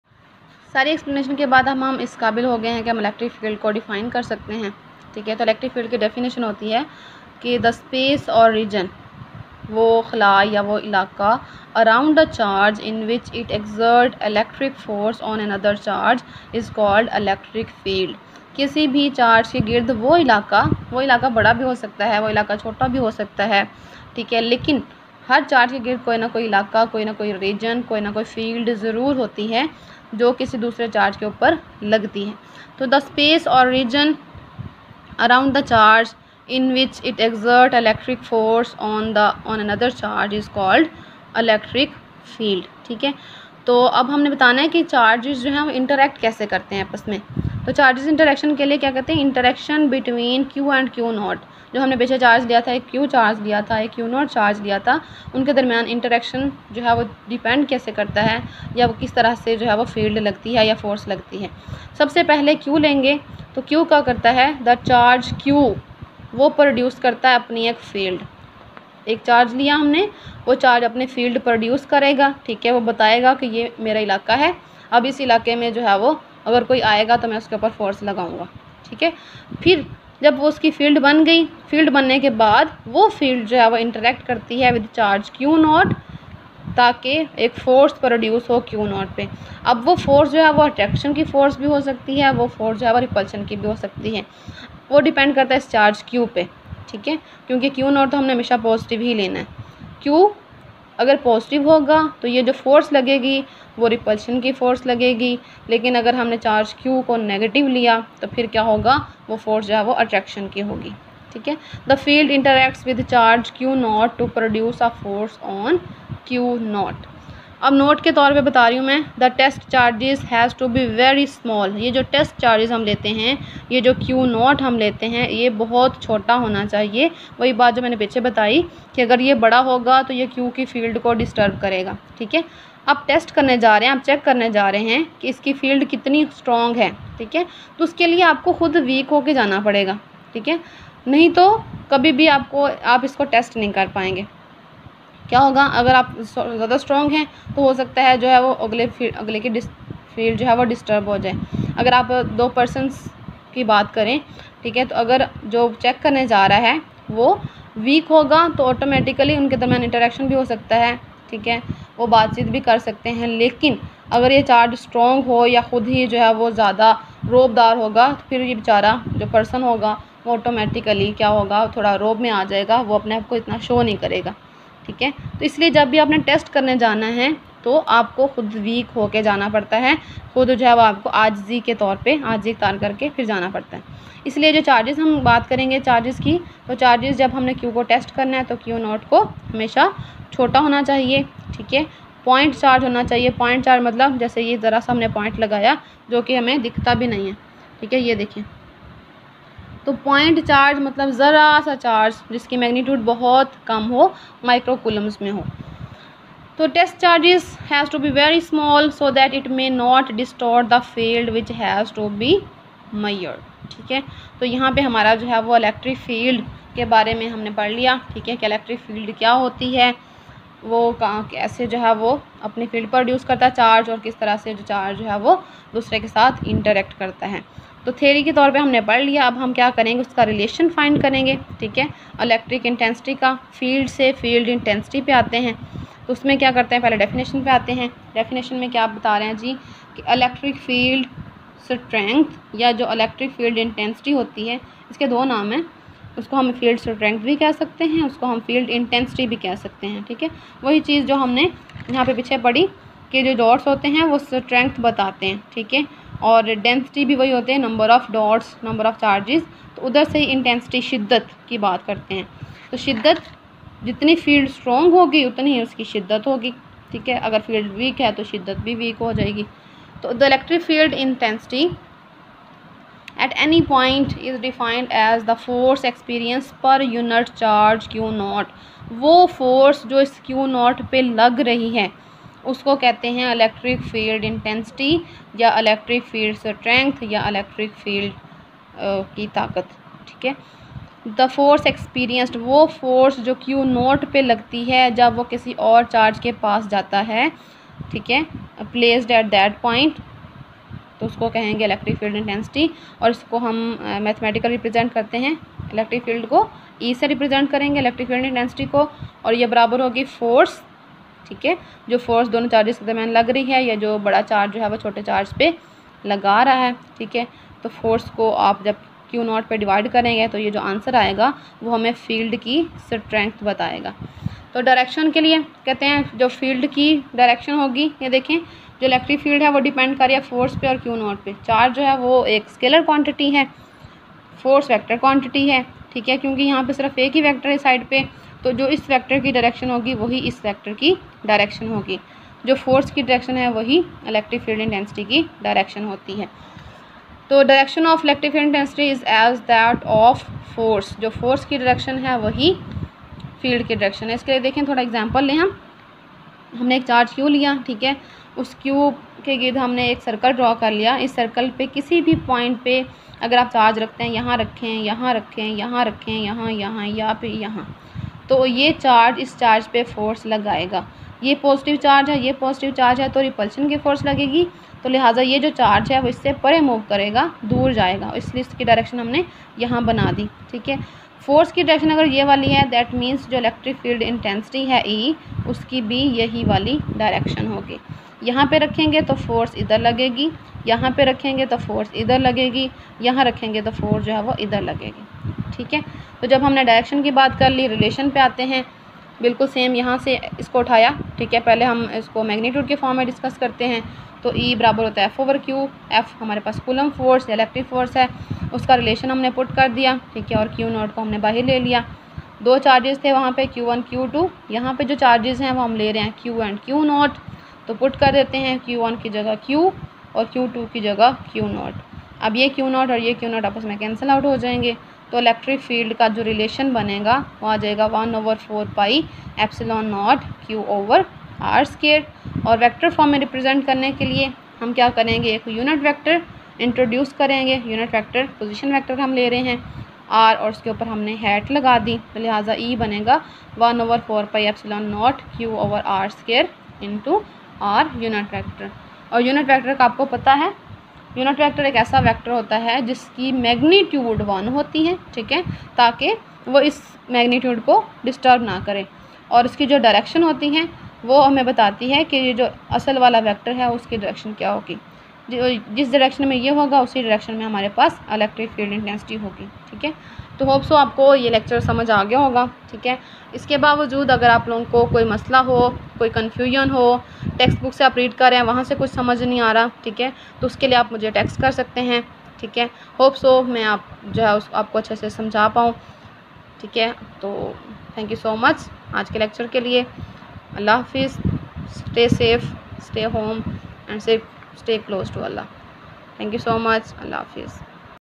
(Topic/questions) Electric field intensity part 2 |PHYSICS PART 2 | CH 12| ELECTROSTATICS
In this vedio you will learn about electric field intensity in detail and you will become able to derive the relation for electric field intensity with ease.
# Love physics